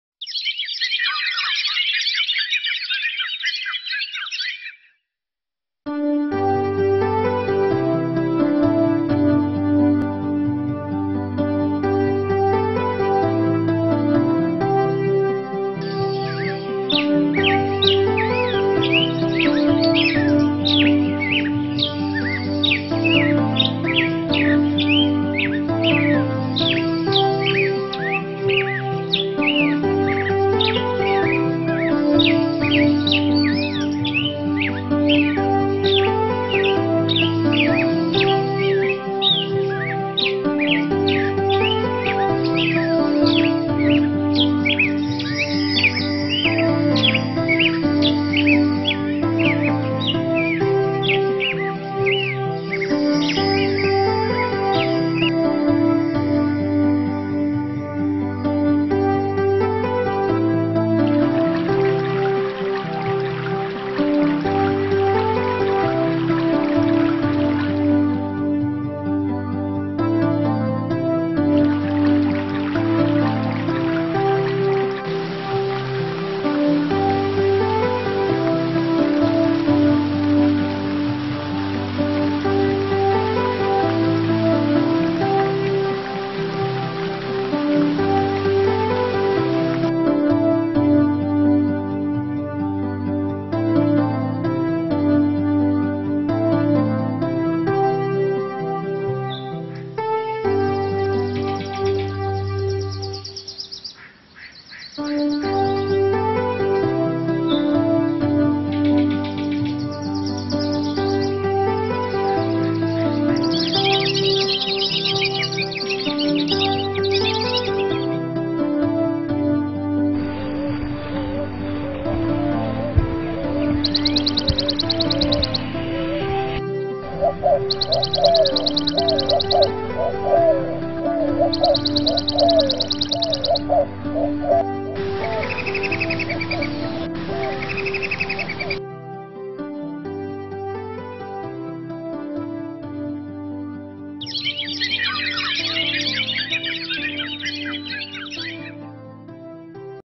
favorite summer.mp4
Some of my favorite summer things, animated nature pictures.